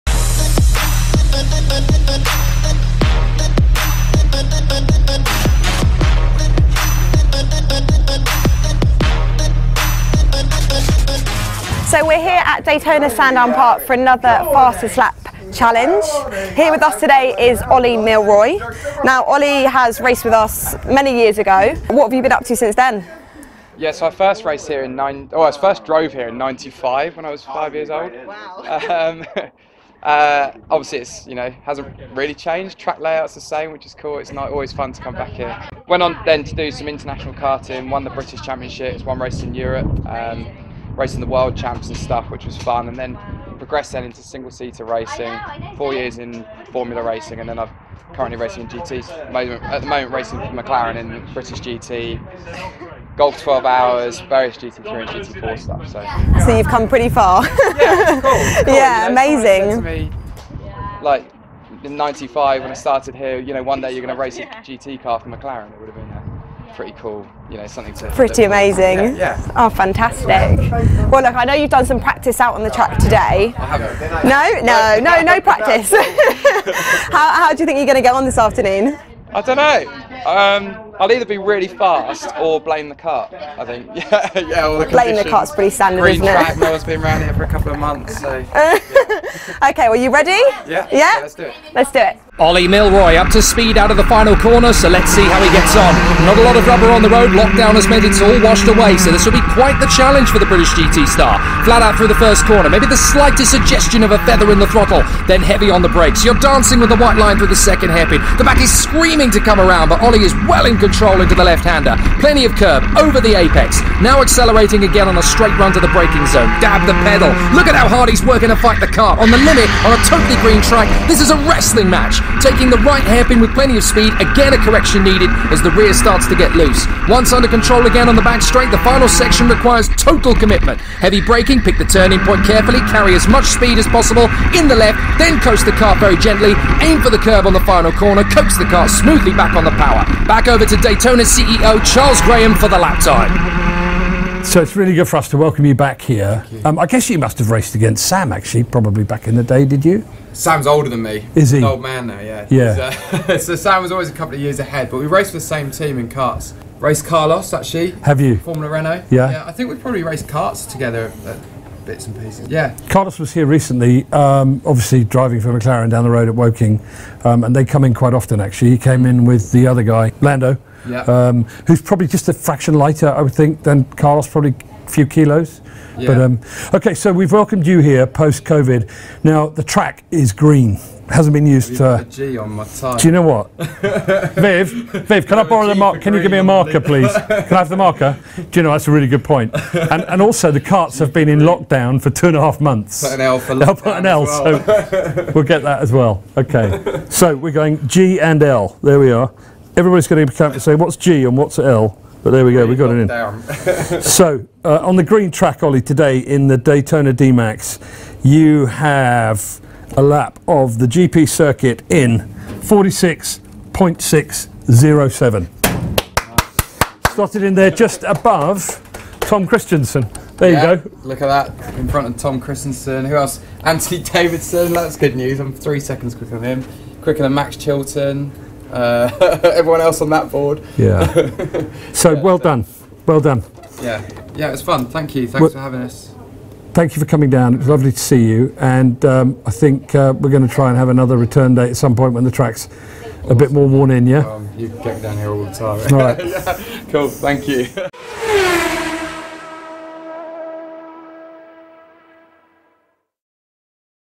OLLIE MILLROY | DAYTONA DMAX FAST LAP CHALLENGE
Racing driver, stunt driver and 2006 British ICA champion Ollie Millroy returns to Daytona Sandown Park to take the DMAX Fast Lap Challenge. Presented by Hannah Salvesen with commentary from Jake Sanson.